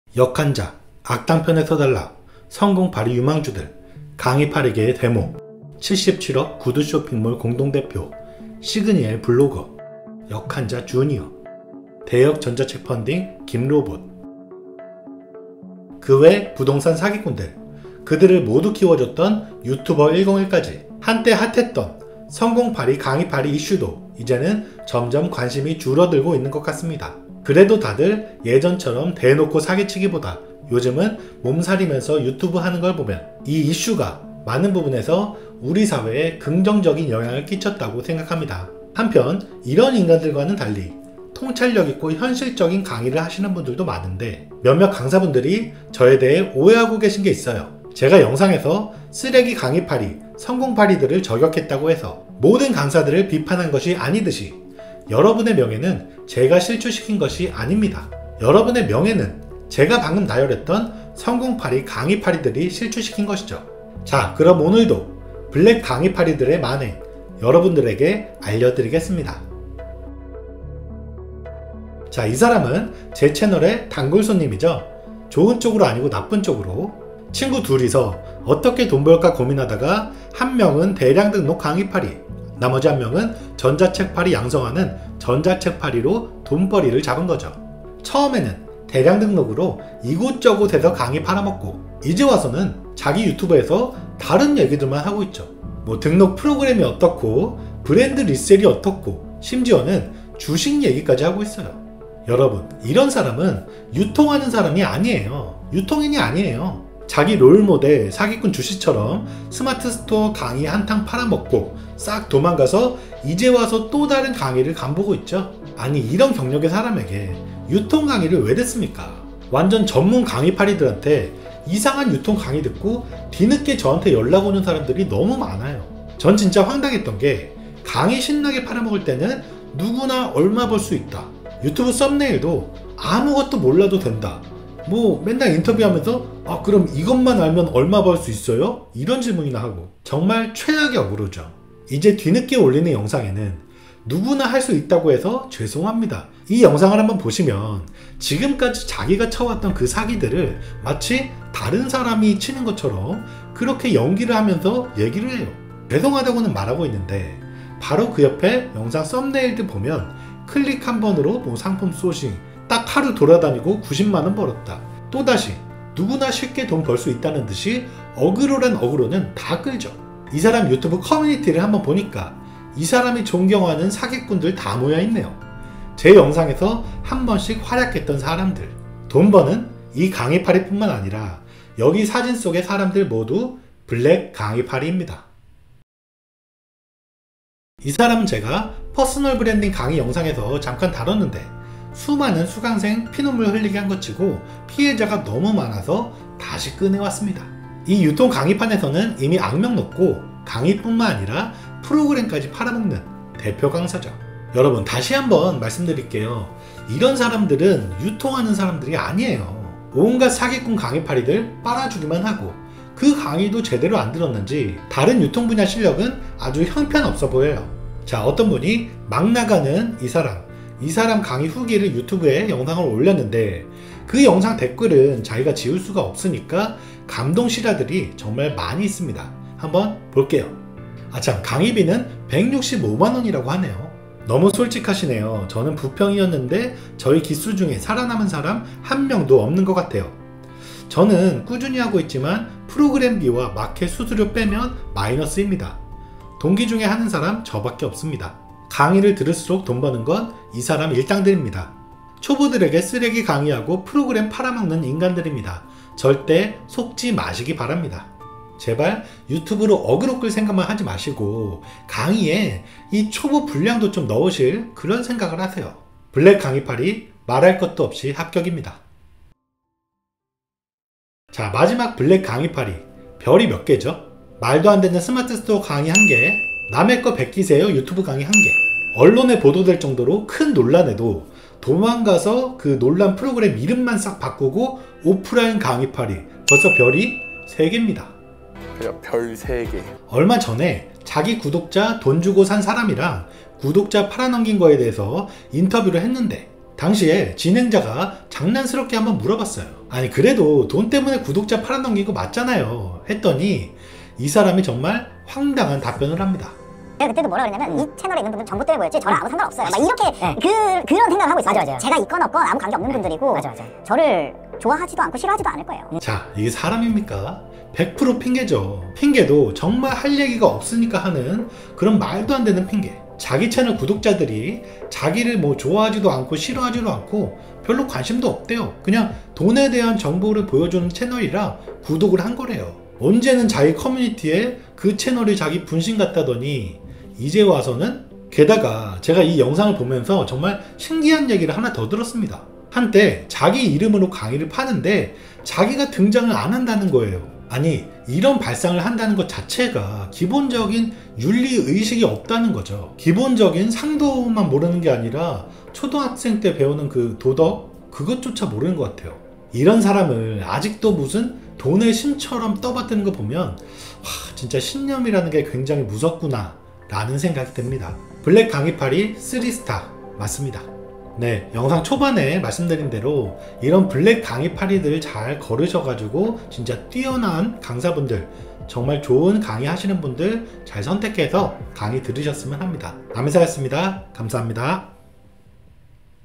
초보 온라인 창업자들 피눈물 나게 한 블랙 강의팔이 싹 잡습니다 : 흑백유통인
남이 사는 브랜드 대표 남이사입니다
사기공화국에서 당하지 않고 사는데 도움이 되셨으면 좋겠습니다
하지만, 비판을 위한 비판은 하지 않습니다

00:00 강의팔이 이슈 이후
01:10 뻔뻔한 죄송합니다
03:06 진짜 막나가는 과장
04:29 블랙 강의팔이 쓰리스타